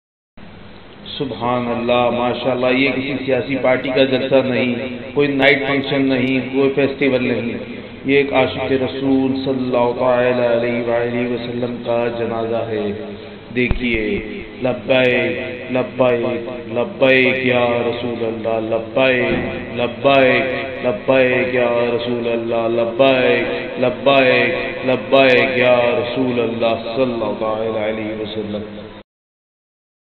Allama Khadim Rizvi K Janazey Ka Fazai Manzar | Minar E Pakistan | Lahore | لبیک یارسول اللہ
Pakistan Lahore